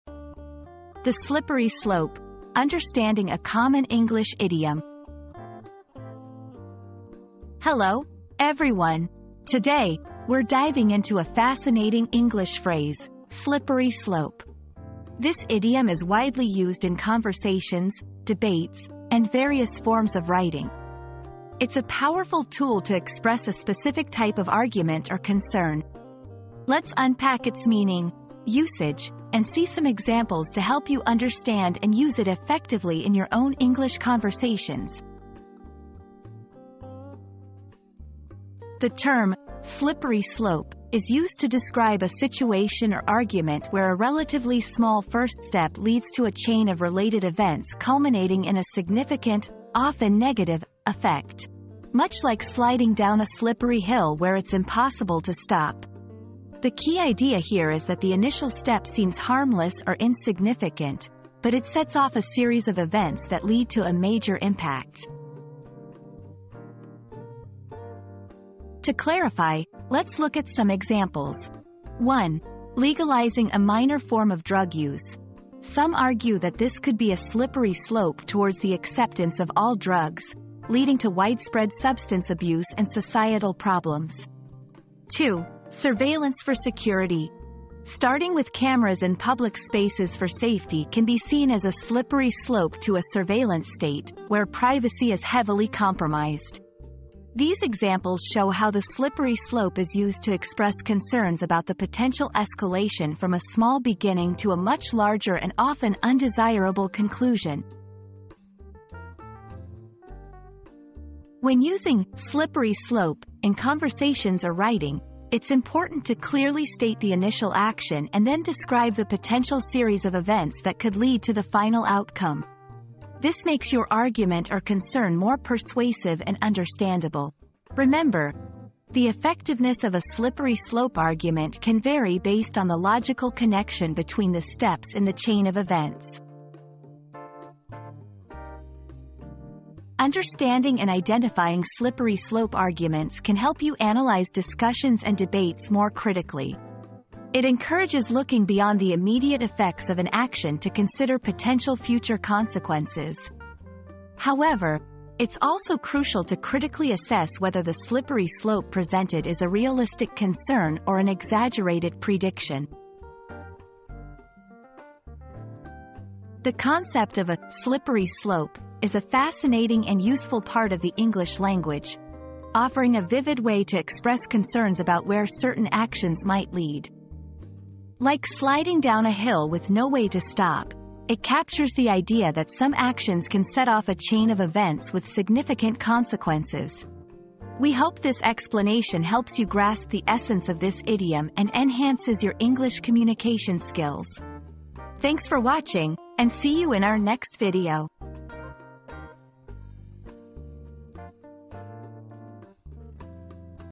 The Slippery Slope: Understanding a Common English Idiom
Mastering the Slippery Slope: Decoding a Popular English Idiom • Unlock the meaning behind the popular English idiom 'The Slippery Slope' and learn how to use it effectively in everyday conversations. Join us as we delve into its origins and explore its various contexts, empowering you to express your thoughts with confidence and clarity.

00:00 • Introduction - The Slippery Slope: Understanding a Common English Idiom
00:38 • What Does "Slippery Slope" Mean?
01:08 • Examples of Slippery Slope
01:50 • How to Use "Slippery Slope" in Conversations
02:19 • The Importance of Recognizing Slippery Slopes